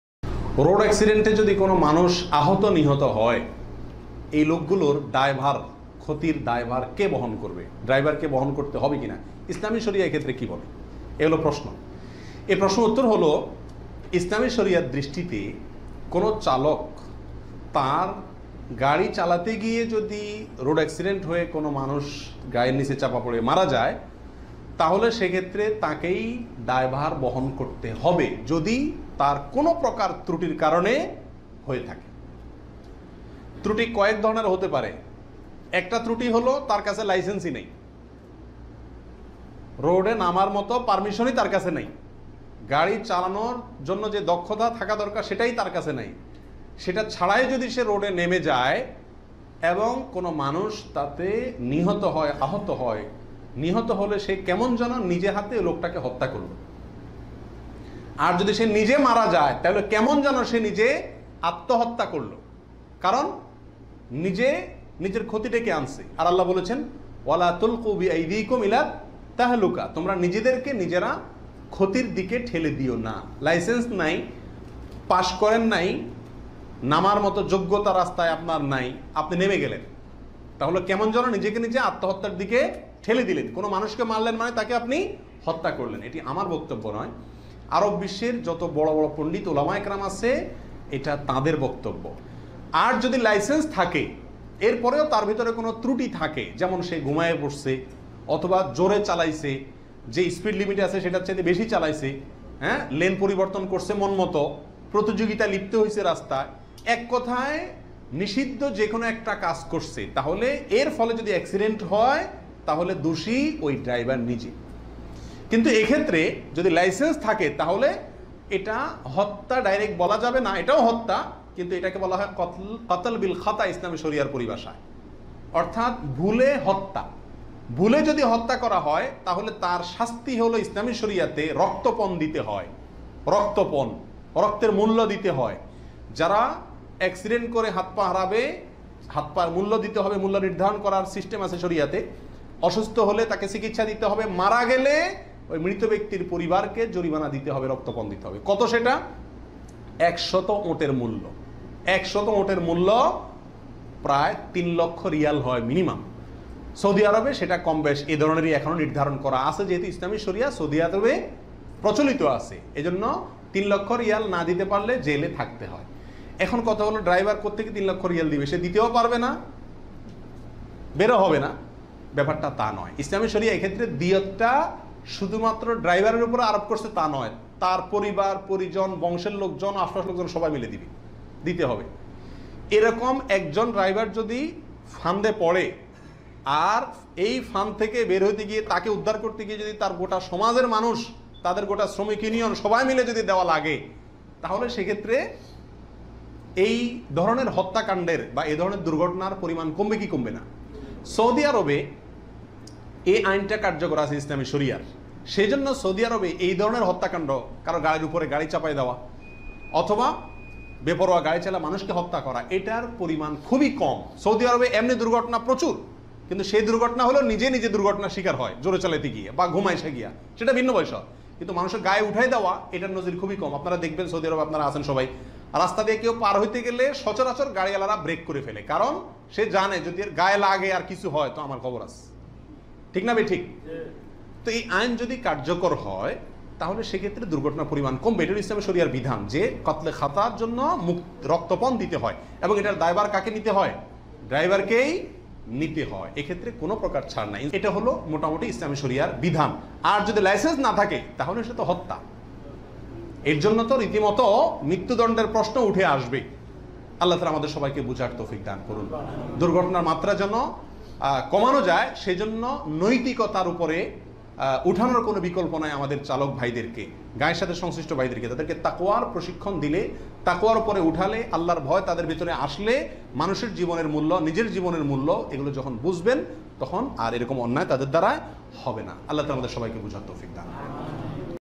সড়ক দুর্ঘটনায় মারা গেলে আত্মহত্যা হবে ? || ইসলামের দৃষ্টিতে সড়ক দুর্ঘটনায় আহতর দায়ভার কার ?
Will he commit suicide if he dies in a road accident ? || In the eyes of Islam, who is responsible for the road accident ?
ইসলামের দৃষ্টিতে সড়ক দুর্ঘটনায় আহত নিহতের দায়ভার কি চালককে নিতে হবে